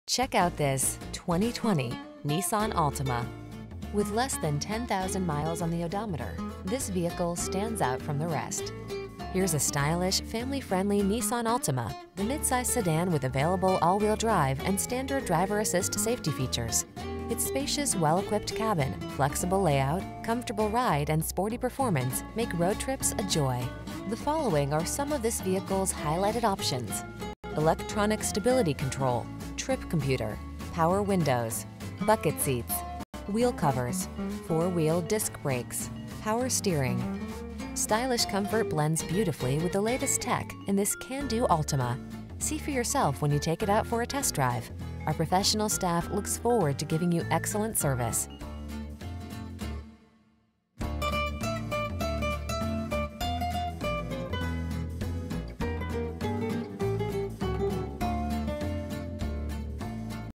2020 Nissan Altima Kennesaw, Marietta, Smyrna, Woodstock, Atlanta, GA PR7178
Deep Blue Pearl Used 2020 Nissan Altima available in Kennesaw, Georgia at Town Center Nissan. Servicing the Marietta, Smyrna, Woodstock, Atlanta, GA area.
2020 Nissan Altima 2.5 S - Stock#: PR7178 - VIN#: 1N4BL4BV2LC283235

2310 Barrett Lakes Blvd NW

Recent Arrival! Priced below KBB Fair Purchase Price! Odometer is 4848 miles below market average!brbrClean CARFAX.brbrDeep Blue Pearl 2020 Nissan Altima 2.5 S FWD CVT with Xtronic 2.5L 4-Cylinder DOHC 16VbrbrCertified.